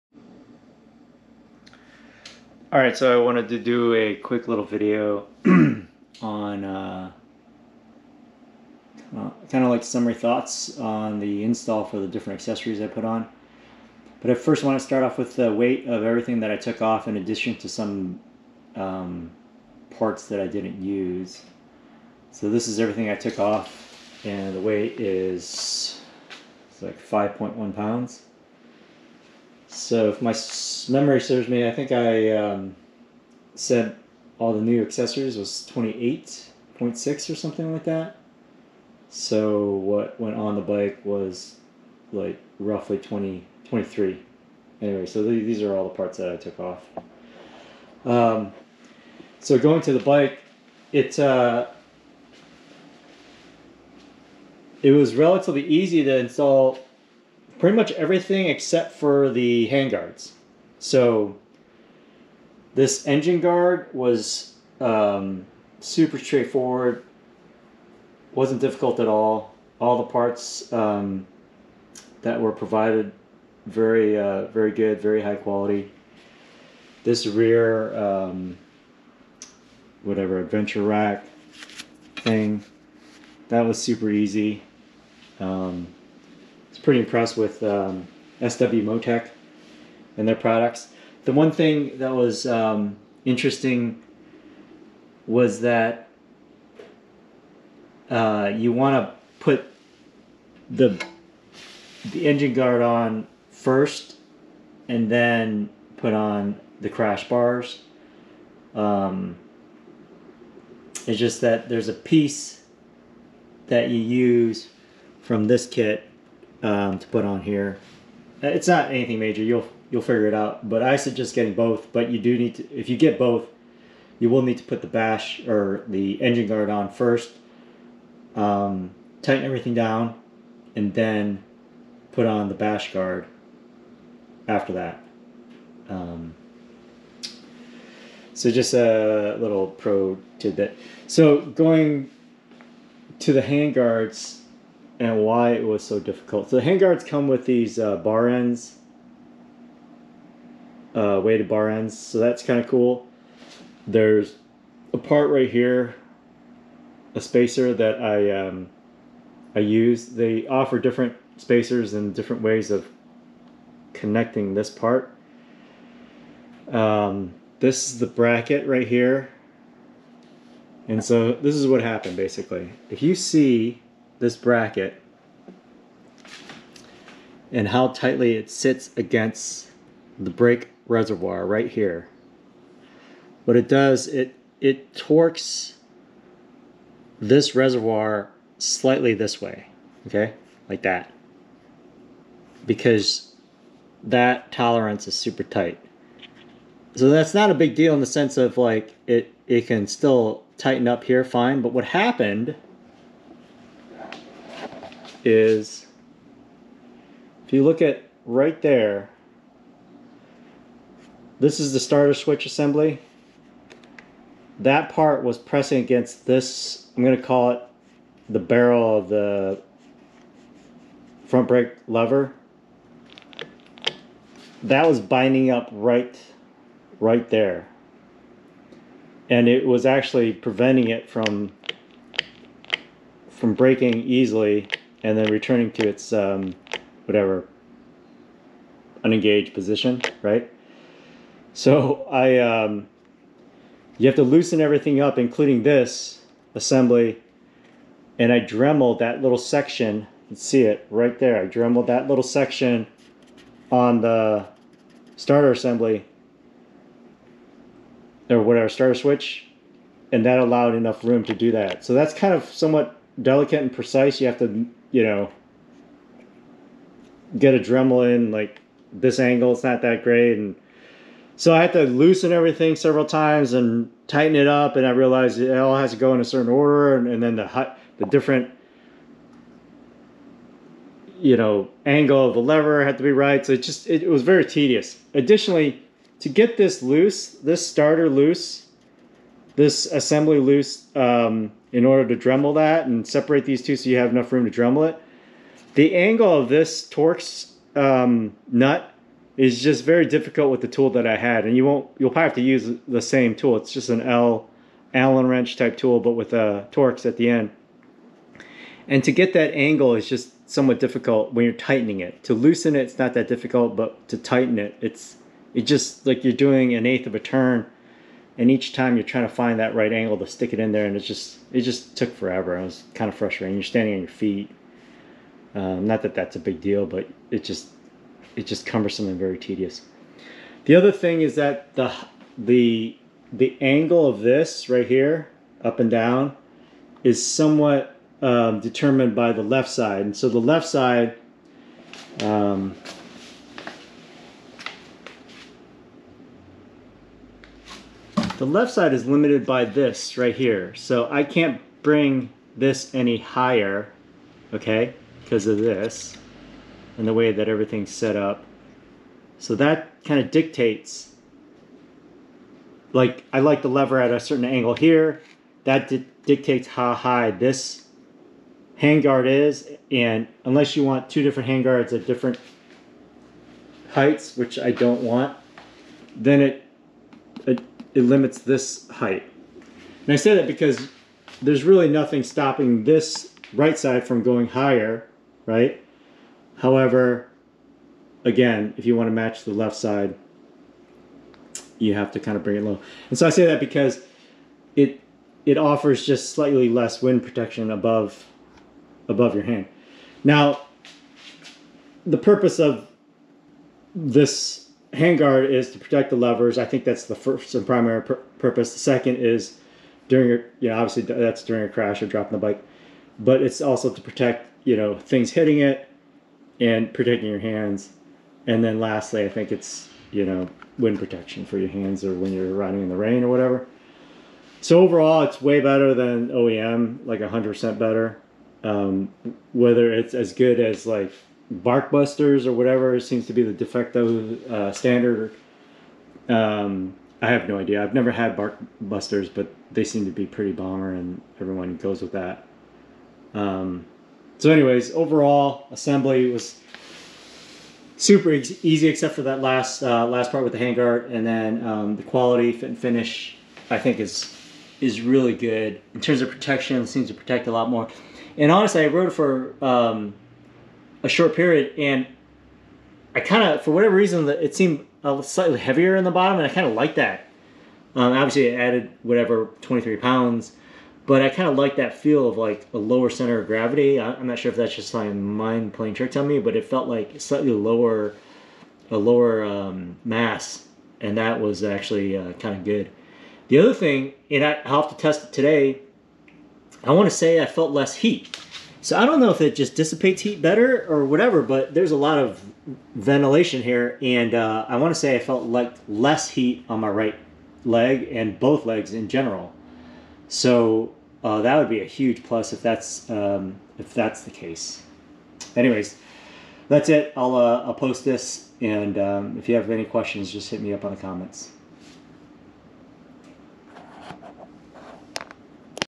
SW-MoTech accessories installation closing thoughts - Aprilia Tuareg 660
Some of my thoughts about the install process of the recent accessories I received.